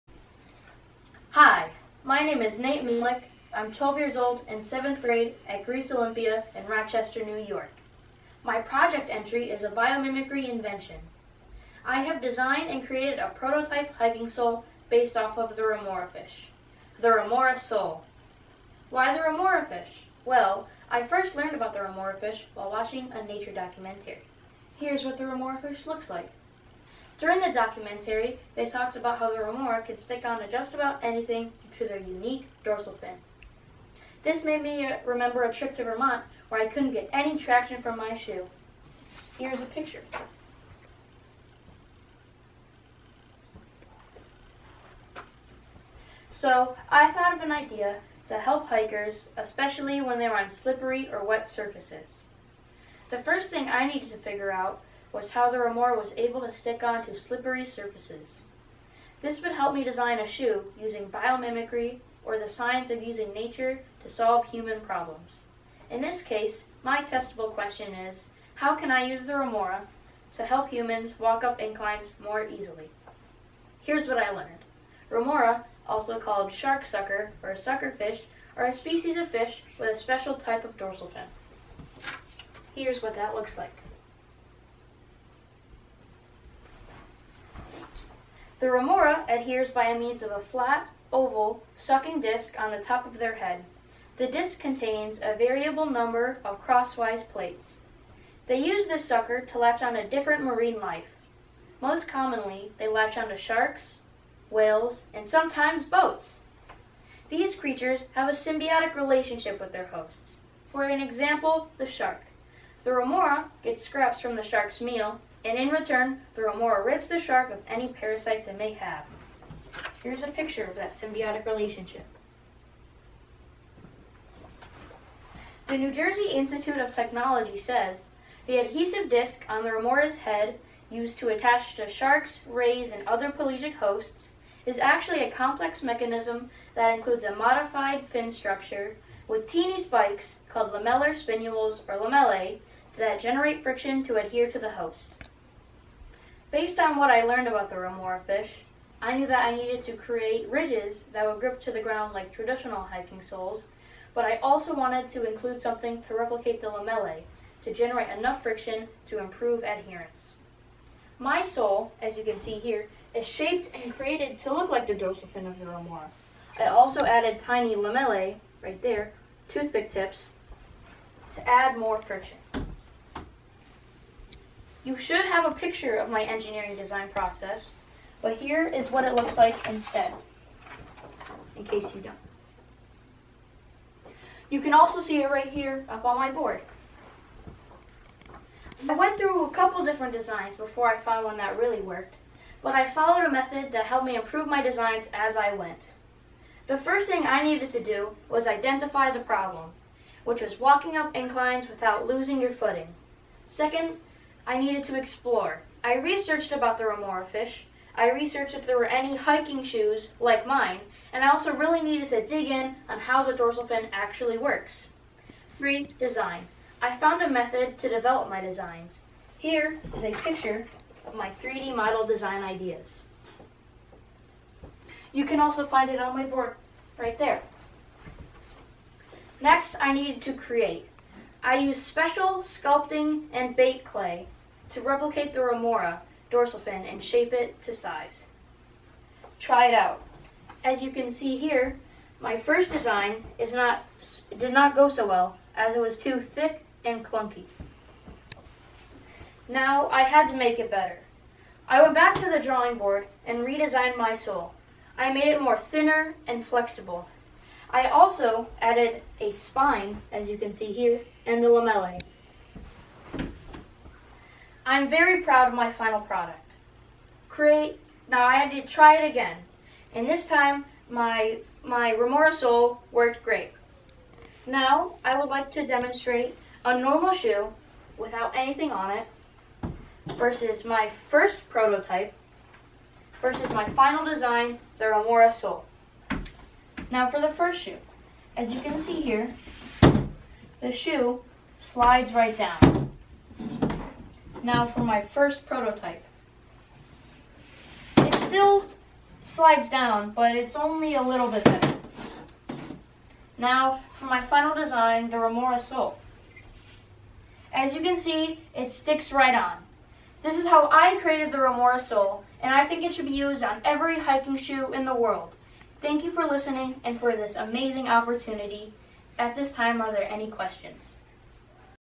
Remora Sole
TRFSEF and Terra virtual science fair presentation, right from our home! Nate presents his ideal hiking sole to offer greater stability while maintaining the ability to walk forward normally on regular terrain. His biomimicry idea is based off of the dorsal fin of a remora fish.